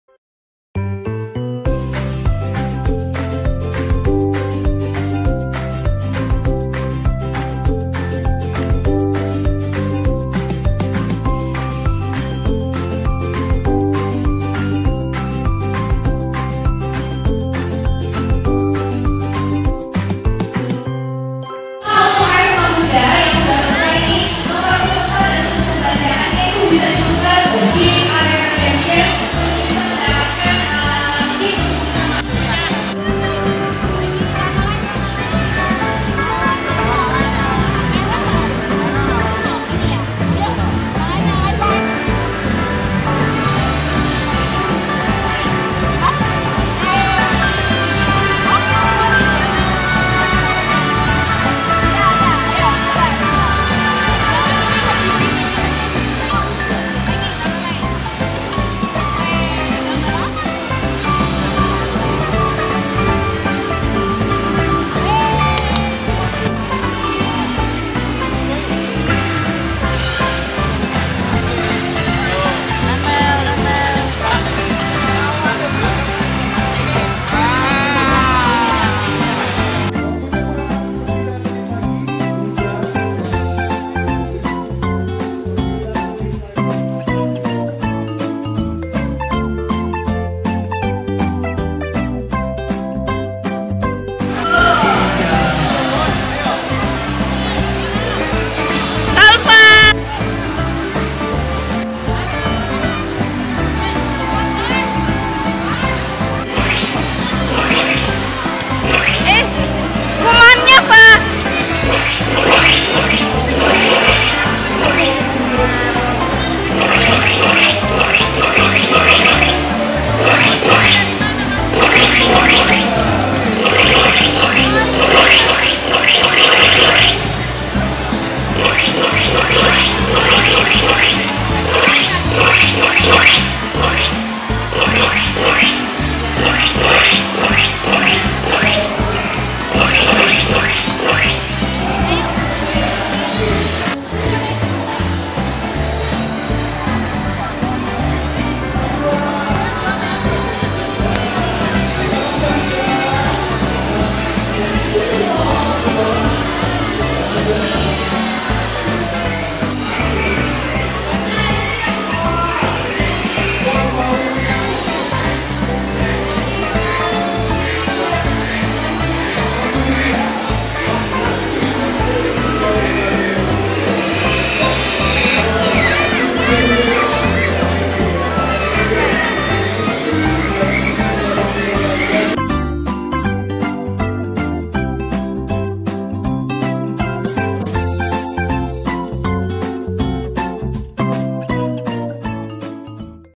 SalfAction #3 - Iya Boleh Camp Dancow - Kids Play at Dancow Playgrounds
Haloo ketemu lagi dengan Salfa, kali ini Salfa bersama bunda menghadiri event IYABOLEH Camp Dancow. Di event IYABOLEH Camp Dancow kali ini Salfa belajar mewarnai pakai jari dan telapak tangan, membuat aneka bentuk dari tanah liat, bermain mandi bola, belajar nyetir mobil, berkuda dan bersama teman-teman yang lain membasmi kuman. Terima kasih sudah menemani Salfa.